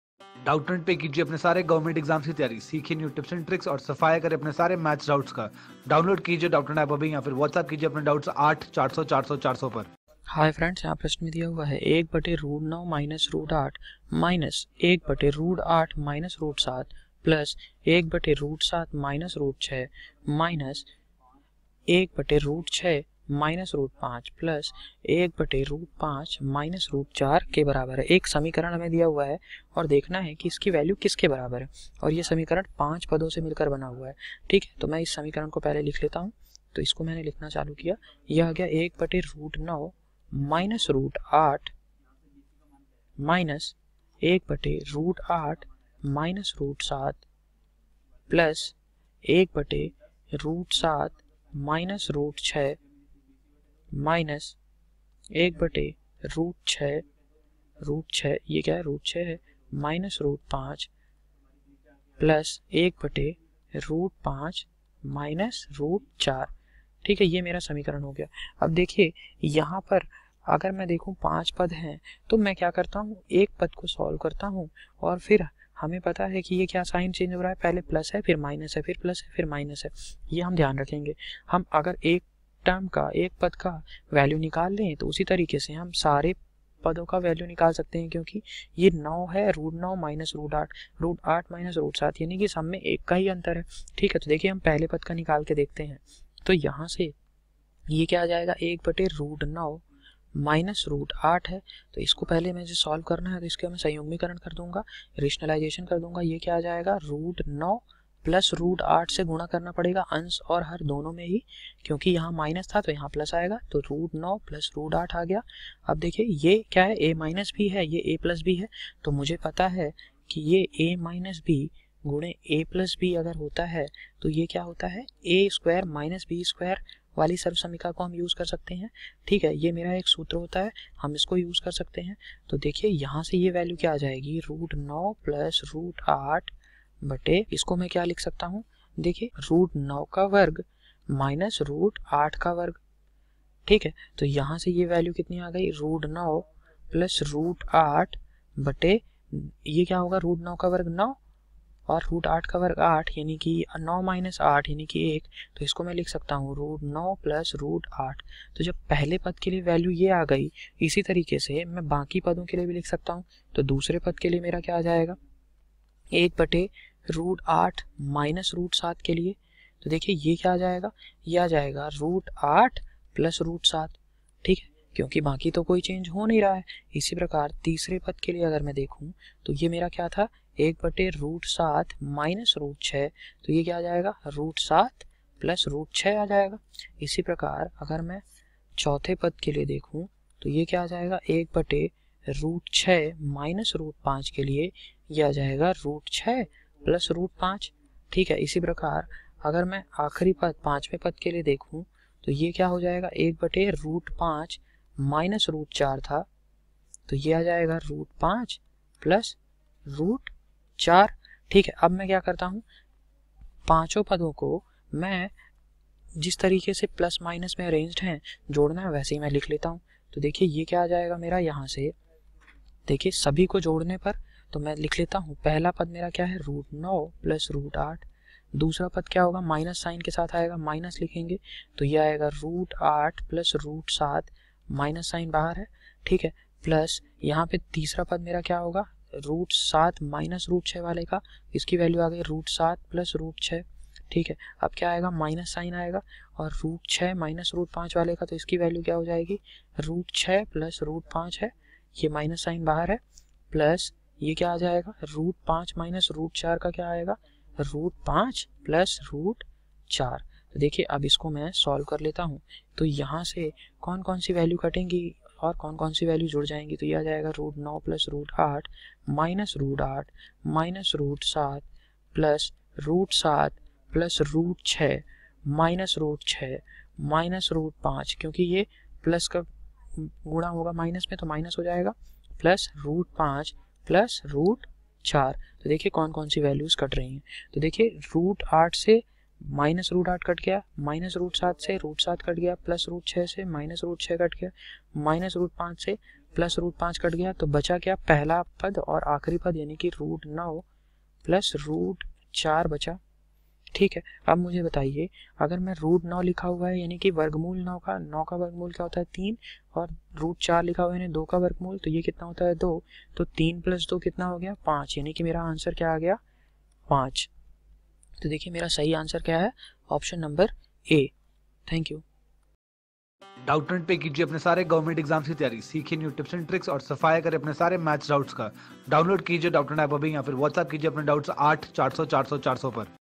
`1/(sqrt(9)-sqrt(8))-1/(sqrt(8)-sqrt(7))+1/(sqrt(7)-sqrt(6))-1/(sqrt(6)-sqrt(5))+1/(sqrt(5)-sqrt...
Question From -  Quantitative Aptitude By Rakesh Yadav Chapter 02 POWER AND INDICES for  Bank PO,SBI PO, IBPS PO,SSC, Railway Entrance & RBI Exam.

QUESTION TEXT:-
`1/(sqrt(9)-sqrt(8))-1/(sqrt(8)-sqrt(7))+1/(sqrt(7)-sqrt(6))-1/(sqrt(6)-sqrt(5))+1/(sqrt(5)-sqrt(4))` ?? ? ???